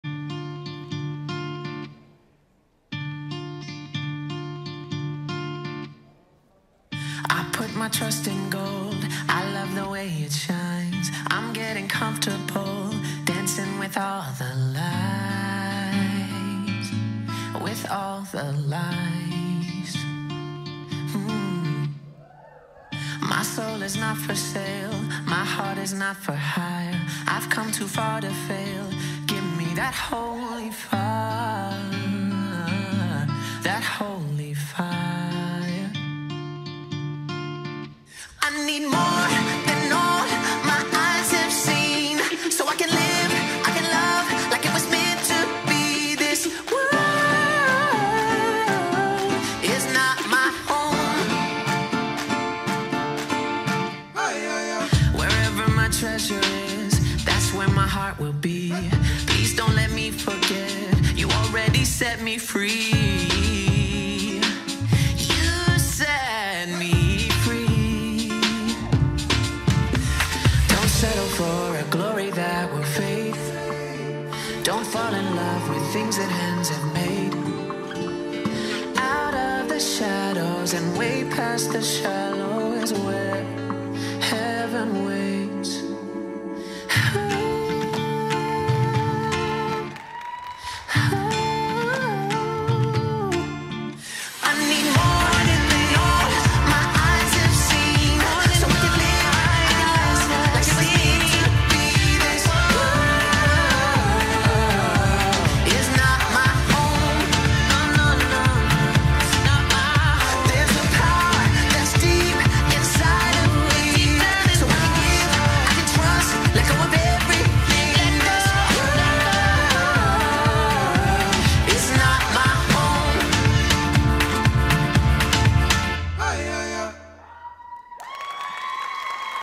Mia Spirit in Motion 2022 This World - Choreography by Kyra Davis for Kennedy Dance Theater
Double Platinum, 2nd Overall and Fabulous Feet award